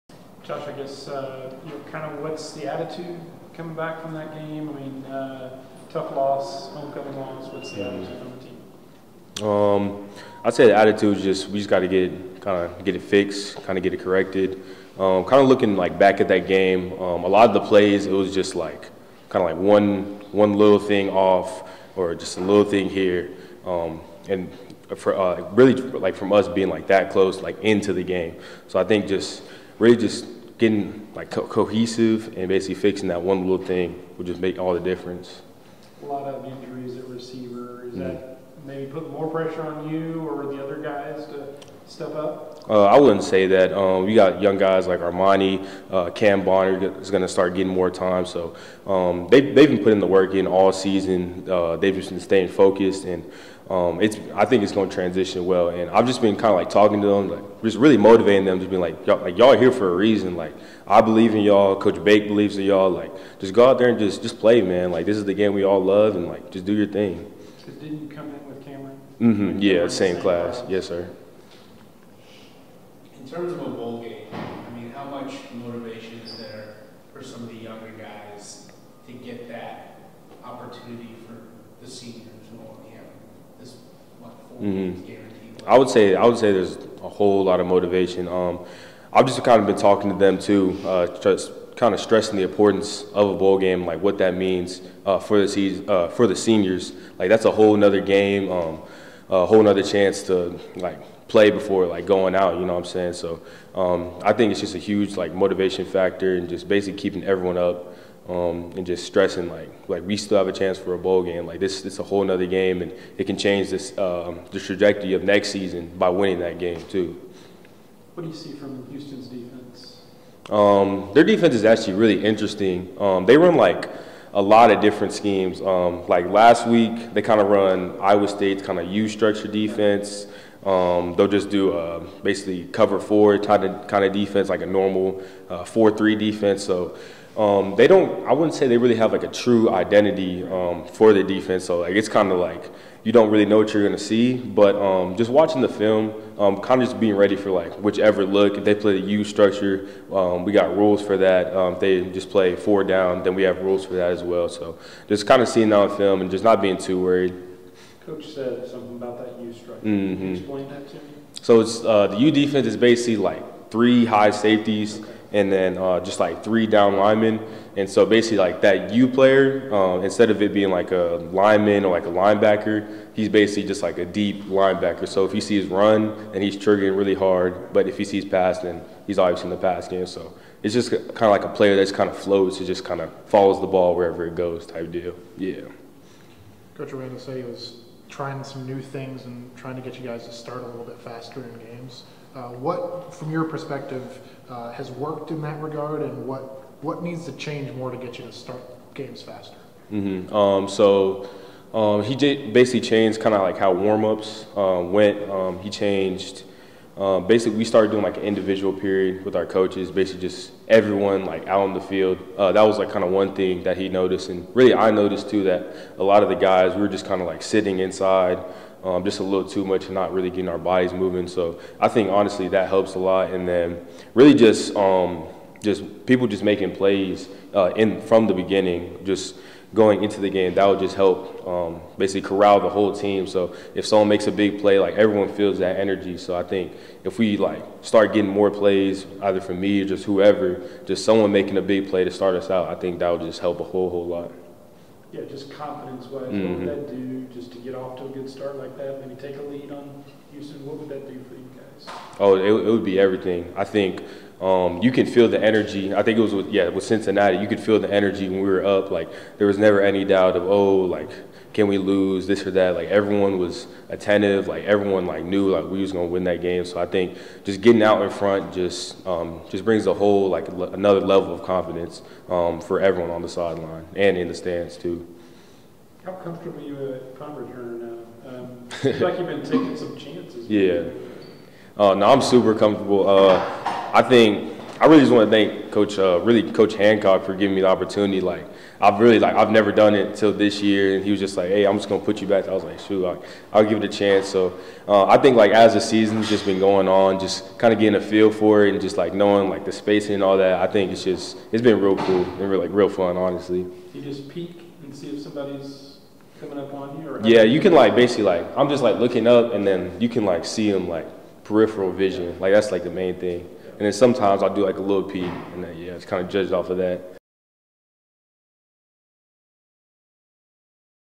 Baylor Football: Josh Cameron Press Conference (October 31, 2023)
Wide receiver Josh Cameron meets with the media ahead of the home matchup versus Houston.